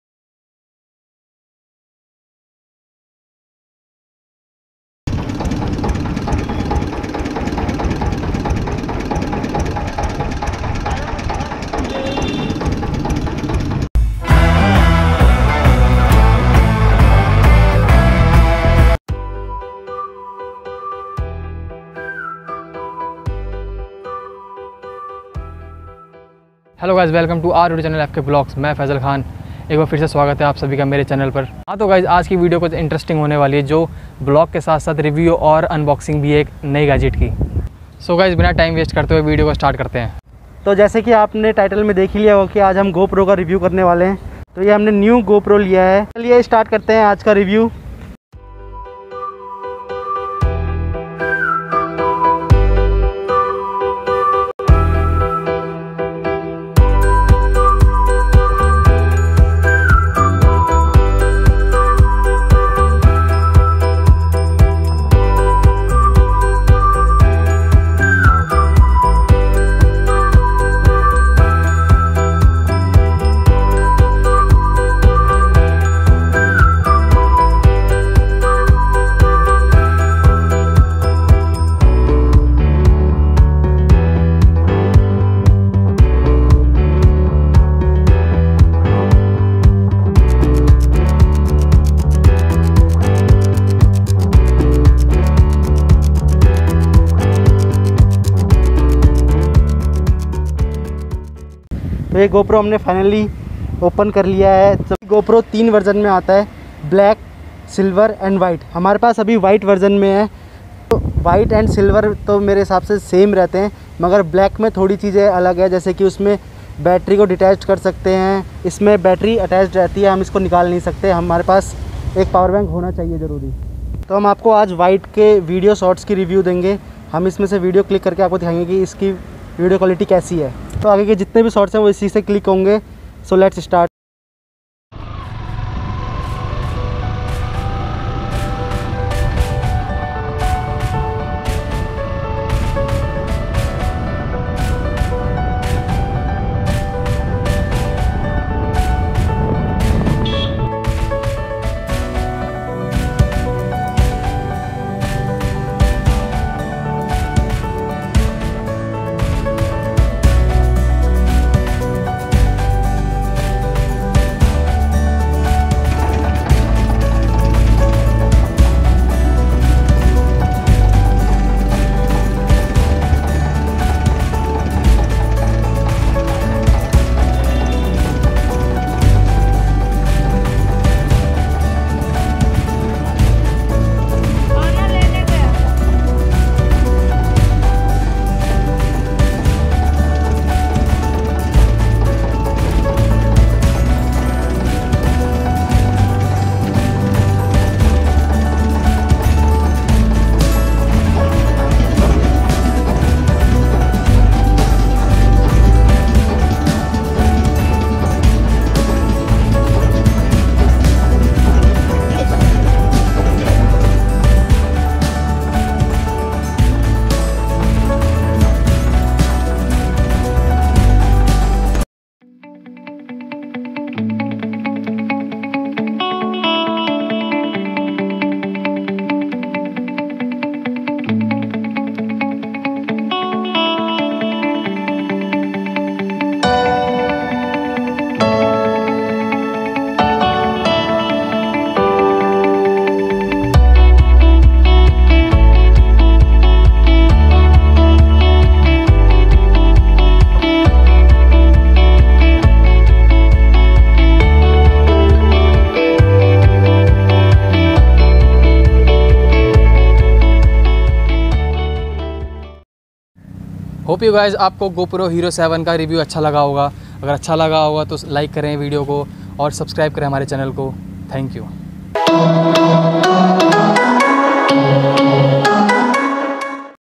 GO PRO 7 UNBOXING // REVIEW // WHITE COLOUR // DWARKA VLOG // DELHI
F.K VLOGS
Here we bring you a new video which is about unboxing of a new gadget in our collection. We will give review, and share our 1st video shot experience with you. Hope you will enjoy the review and vlog.